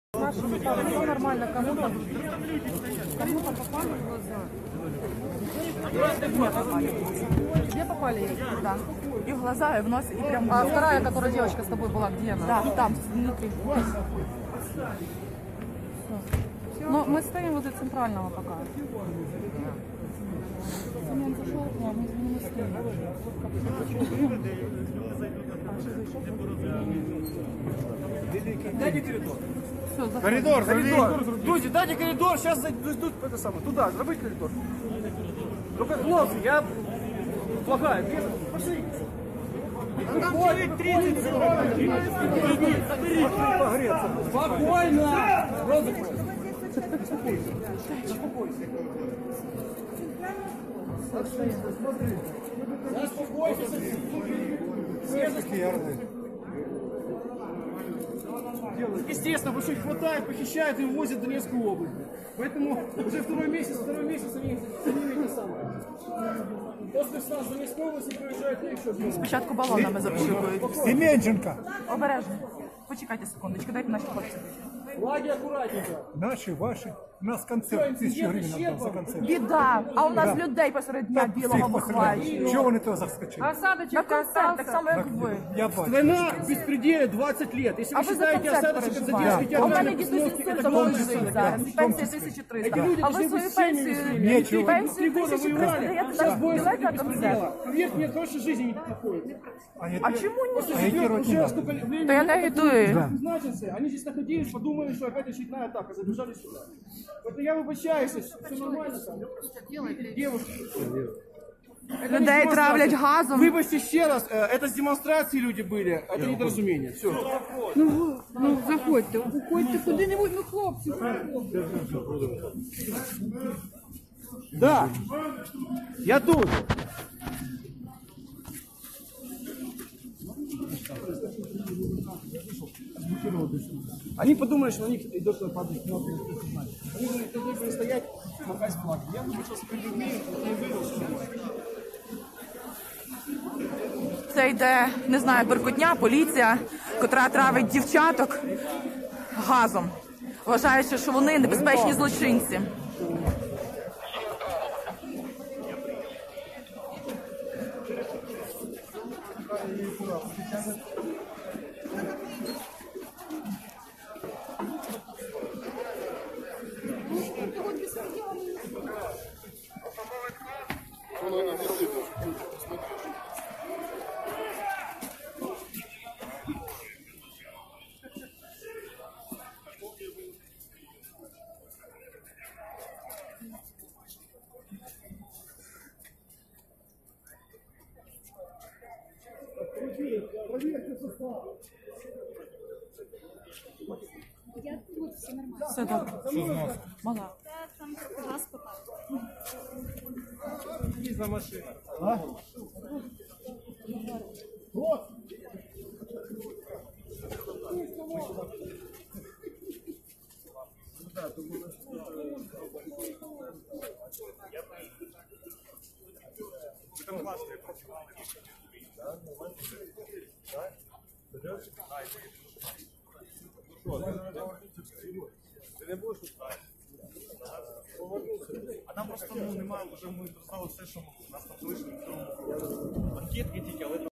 Щойно під Жовтневим палацом у Києві
Рух "Визволення": Щойно під Жовтневим палацом у Києві близько сотні поліцейських розприскали перцевий газ на групу активістів, які проводять протест за звільнення наших ув'язнених побратимів.